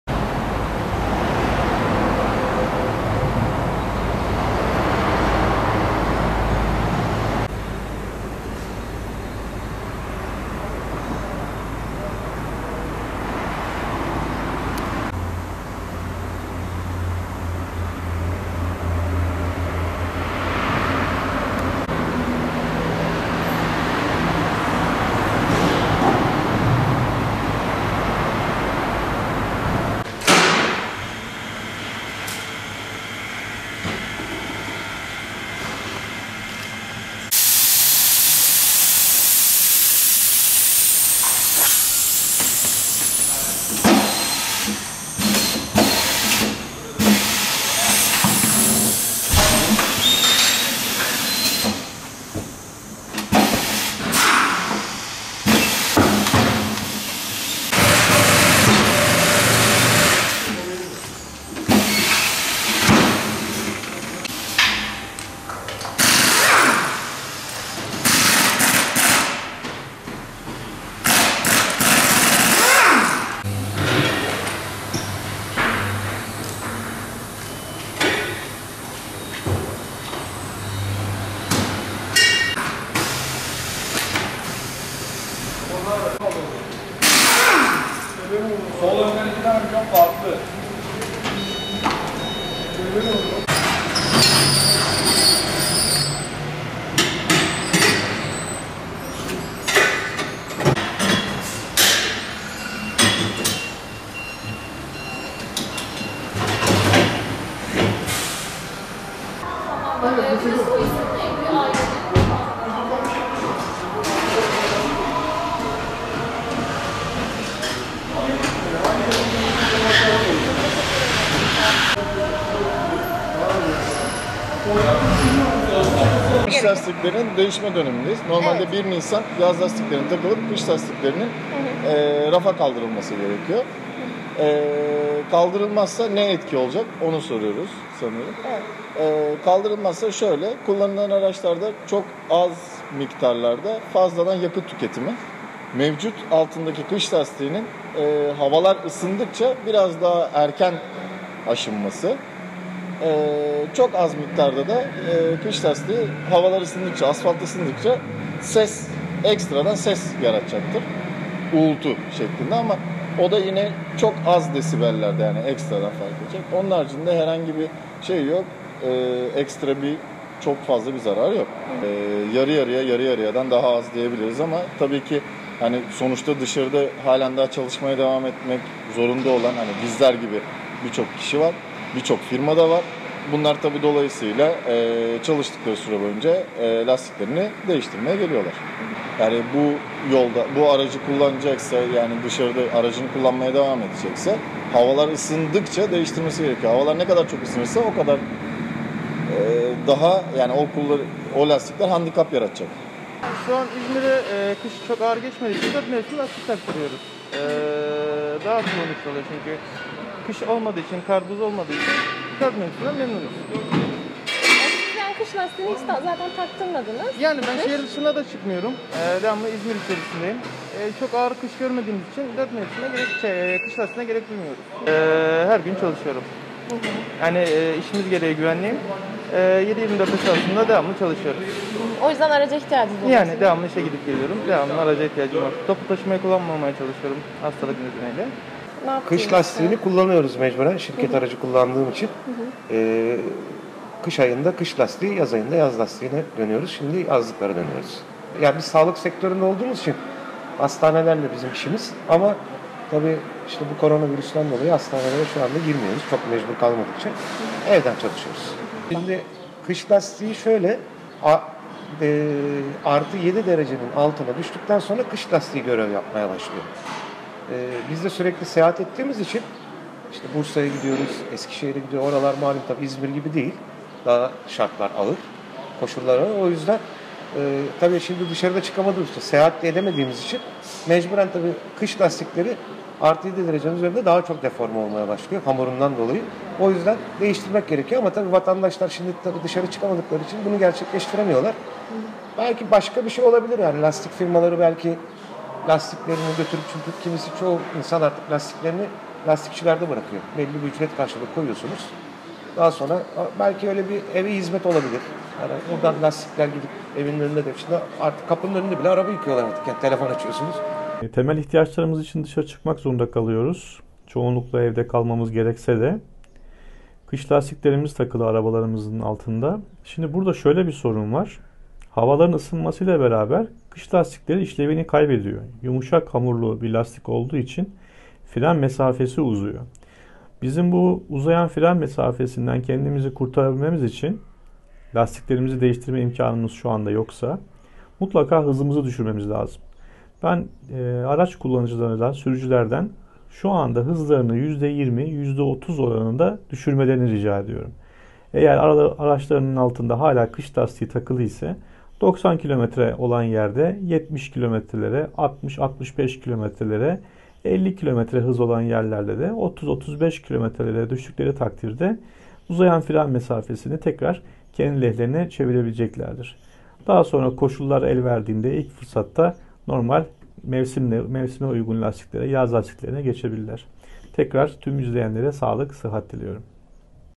Kış lastiklerini değiştiremeyenlere uyarı: Hızınızı düşürün
Ticari araçlarda kış lastiği zorunluluğu 1 Nisan'da sona erdi. Fakat koronavirüs salgını nedeniyle evlerinden çıkmayan vatandaşlar lastiklerini henüz değiştirmedi. İleri Sürüş Teknikleri Uzmanı Melih Himmetoğlu, şartlar nedeniyle lastiklerini değiştiremeyenleri uyararak, "Yumuşak hamurlu bir lastik olduğu için fren mesafesi uzuyor. Lastikleri değiştirme imkanları yoksa hızı yüzde 20, yüzde 30 oranında düşürmelerini öneriyorum" dedi.

Ticari araçlarda, Aralık'tan itibaren 4 ay boyunca kullanılması zorunlu olan kış lastiğinin süresi, hava sıcaklıklarının da yükselmesiyle 1 Nisan'da sona erdi. Fakat salgın nedeniyle evlerinden çıkmayan vatandaşlar araçlarını kullanmadıkları için henüz yaz lastiğine geçiş yapmadı. Salgın günlerinde çalışan ve her gün araç kullanmak zorunda olan kişiler ise, soluğu oto lastikçilerde aldı. İzmir'de, bir oto lastik şirketinde satış temsilciliği yapan Zekai Timurkoçan, havalar ısındığında kış lastiklerinin değiştirilmesi gerektiğini hatırlatarak, aksi durumda araçlarda fazla yakıt tüketimine, lastiklerin erken aşınmasına ve uğultuya sebebiyet verebileceğini söyledi.

FAZLA YAKIT TÜKETİMİ, AŞINMA VE UĞULTU

Kış lastiklerinin değişme döneminde olduklarını anlatan Timurkoçan, "Normalde 1 Nisan'dan itibaren kış lastiklerinin rafa kaldırılması gerekiyor. Değişim yapılmazsa araçlarda az da olsa fazladan yakıt tüketimi, mevcut kış lastiğinin havalar ısındıkça aşınması ve uğultu yapması gibi sorunlar ortaya çıkabilir. Bunlar büyük zararlar değil, fakat dikkatli olmak gerekir. Bu sene lastiklerini değiştirmeye gelenlerin sayısı geçen seneye göre yüzde 50 daha düşük. Fakat bizler gibi çalışmak zorunda olan kişiler de var. Birçok firmanın araçları her gün çalışıyor, onlar lastikleri değiştirmeye geliyorlar. Eğer araç sürekli kullanılıyorsa, havalar ısındıkça bu lastiklerin değişmesi için kalan süre kısalıyor. Hava sıcaklığı ne kadar yükselirse, kış lastikleri o kadar fazla sorun yaratır" dedi.

'MUTLAKA HIZI DÜŞÜRMEMİZ LAZIM'

Şartlar nedeniyle kış lastiklerini değiştiremeyenlere tavsiyelerde bulunan İleri Sürüş Teknikleri Uzmanı Melih Himmetoğlu ise, "Çoğunlukla evde kalmamız gerekse de temel ihtiyaçlarımız için dışarı çıkmak zorunda kalıyoruz. Çoğumuzun arabasında da hala kış lastiği takılı. Havaların ısınmasıyla kış lastikleri işlevini kaybediyor. Yumuşak hamurlu bir lastik olduğu için fren mesafesi uzuyor. Uzayan fren mesafesinden kendimizi kurtarabilmemiz için, lastikleri değiştirme imkanı yoksa mutlaka hızı düşürmemiz lazım. Kış lastiklerini değiştiremeyen sürücülere hızlarını yüzde 20 ila 30 oranında düşürmelerini öneriyorum. Eğer araçlarının altında kış lastiği takılı ise, hız sınırı 90 km olan yerlerde 70 km hızla, sınırı 50 km olan yerlerde ise 30-35 km hızla gitmeleri gerekir. Bu şekilde uzayan fren mesafesini kendi lehlerine çevirebilirler. Koşullar el verdiğinde, ilk fırsatta yaz lastiklerine geçebilirler" şeklinde konuştu.

'İŞE GİDİP GELMEK ZORUNDAYIM, LASTİKLERİ DEĞİŞTİRDİM'

İşe gidip geldiği için aracının lastiklerini değiştiren Mahmut Taşçı, "İzmir'de kış çok sert geçmediği için 4 mevsim lastik kullanıyorum. Bir şirketin güvenlik departmanında çalışıyorum. O yüzden 7/24 iş gidip gelmek durumundayım. Bu nedenle aracımı her gün kullanıyorum" diye konuştu.

Ticari araç kullanan Gökhan Paker ise, "Şirket aracı kullandığım için mecburen kış lastiği kullanıyorum. Yaz aylarında yaz lastiğine dönüyorum. Kendi aracımın lastiklerini değiştirdim. Sağlık sektöründe çalışıyorum, hastanelerle çalışıyoruz. Koronavirüs nedeniyle hastanelere gitmiyoruz, mecbur kalmadıkça. O yüzden genelde evden çalışıyorum. Hava sıcaklığı 7 derecenin altına düştükten sonra kış lastiği görev yapmaya başlıyor. Ben şehir dışına, daha soğuk bölgelere de gittiğim için işime yarıyordu. Fakat şimdi sıcaklıklar 7 derecenin çok üstünde olduğu için lastikler deforme oluyor. Değiştirmek gerekiyor. Dışarı çıkamayanlar bunu gerçekleştiremiyor. Belki lastikçiler evlere hizmet yapabilir. Lastikler ev önlerinde değiştirilebilir" dedi.